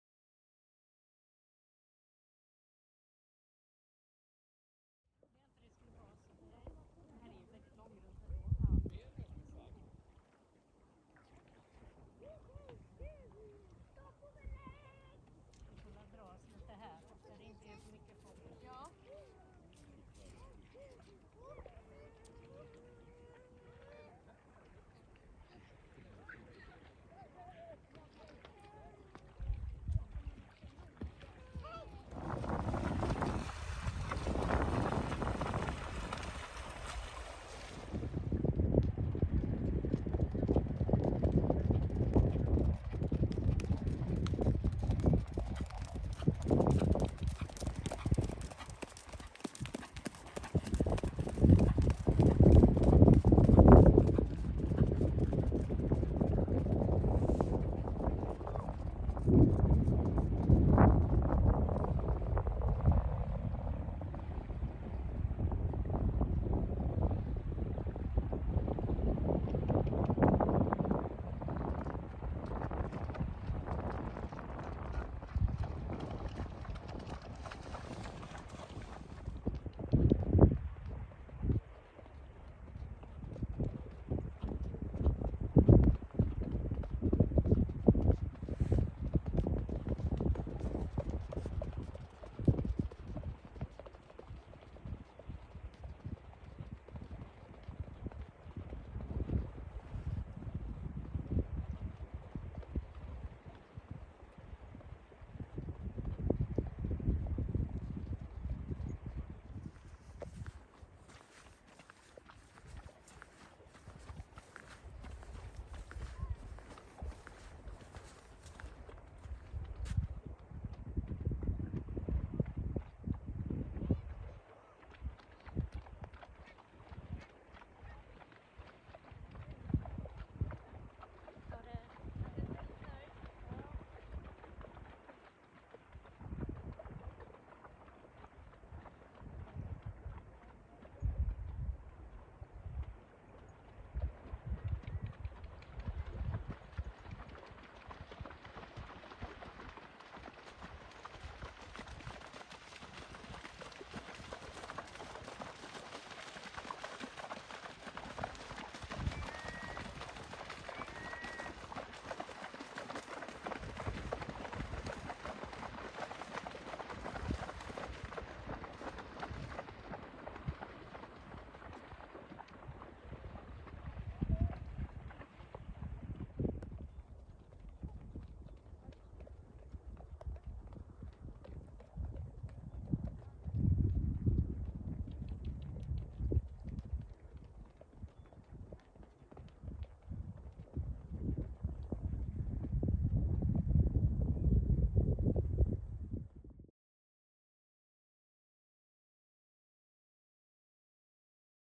Valur och Silver-Stjärna på stranden
1 september får man äntligen rida på stranden igen så vi red ner med Valur och Silver-Stjärna till Björkäng. Det var ganska mycket folk som badade så vi red lite lugnt på norra delen av stranden.